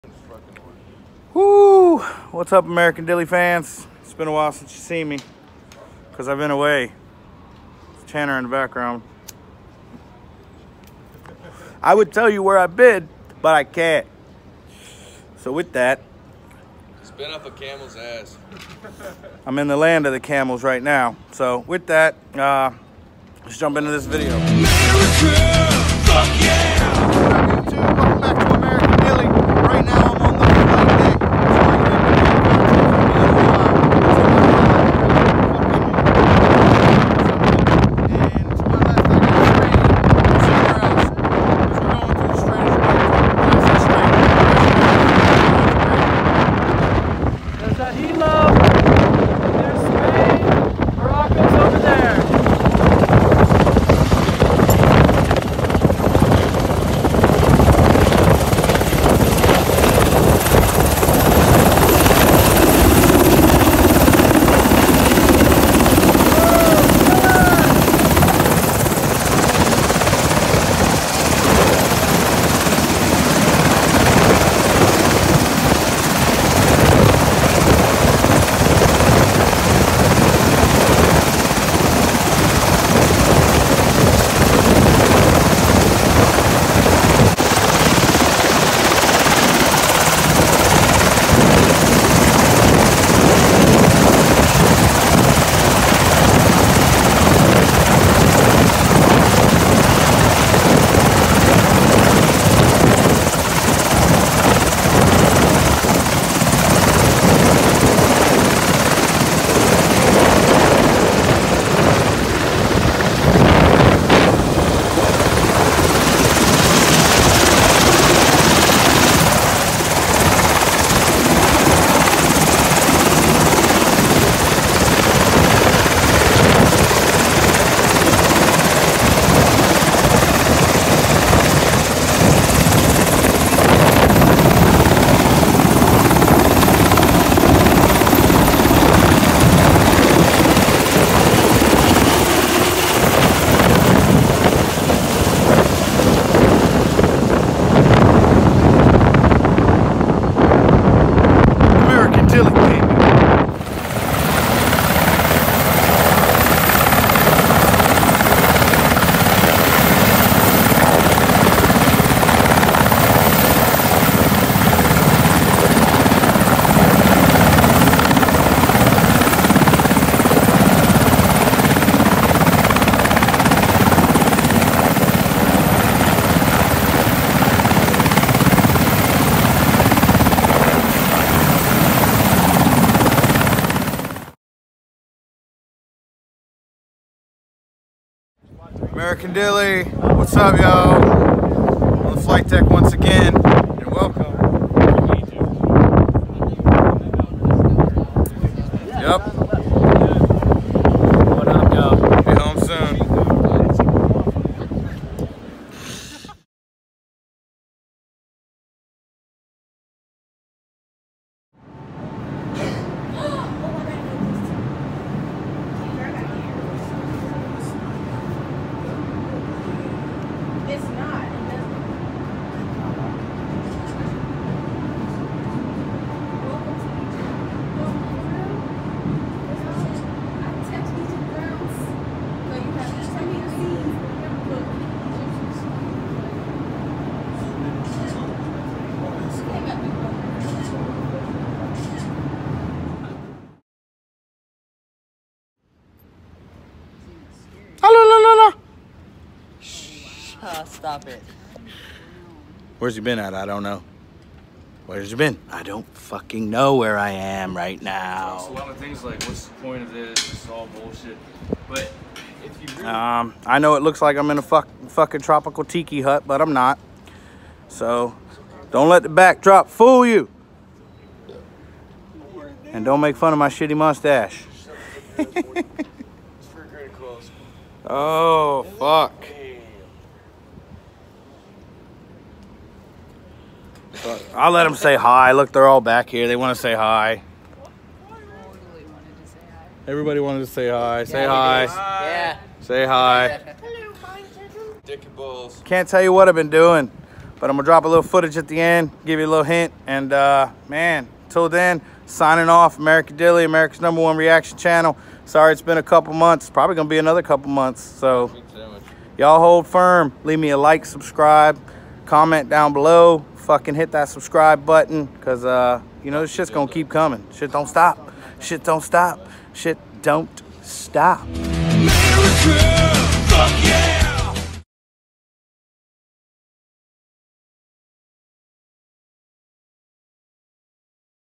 Where In The World Is AMERICAN DILLY - The Navy Experience Life aboard an Aircraft Carrier
Where in the world is American Dilly? Deployment life is rough but we make it look so easy!